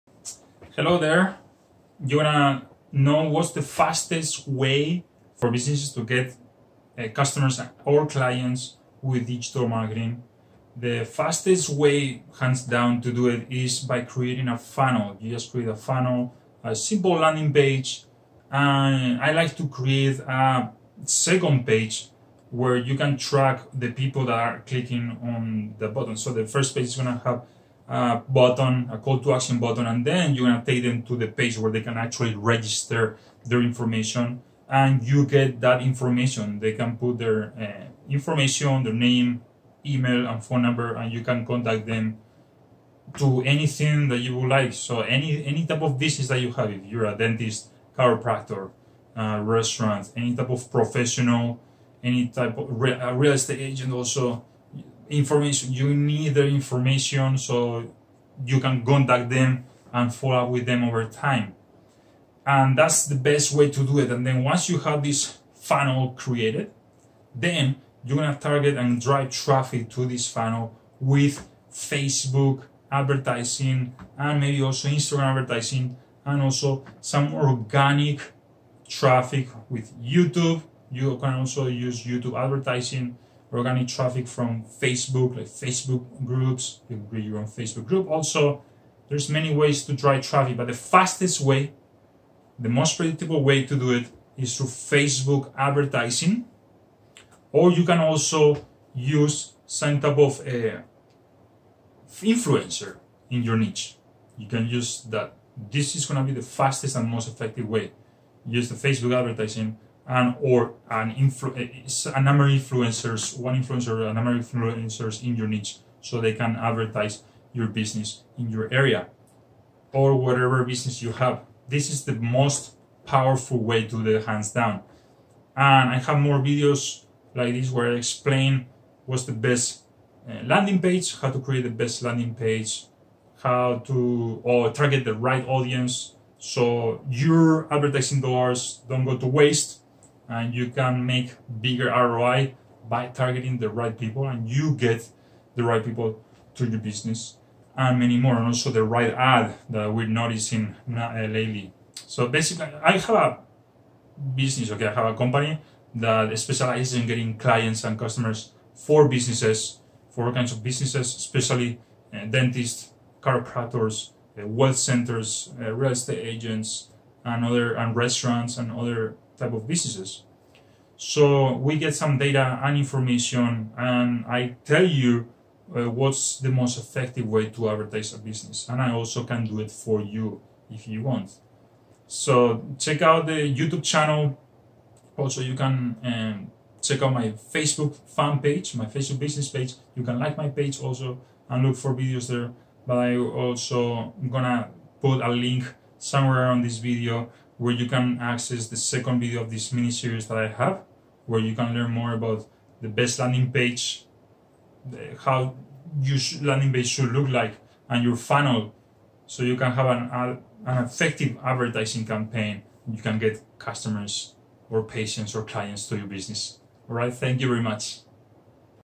The Fastest Way To Get Customers With Digital Marketing (Part 1 of 4)
What's the fastest way to get customers with digital marketing?

Watch this video to find out the exact strategy that the most successful businesses use to get the most amount of customers FAST.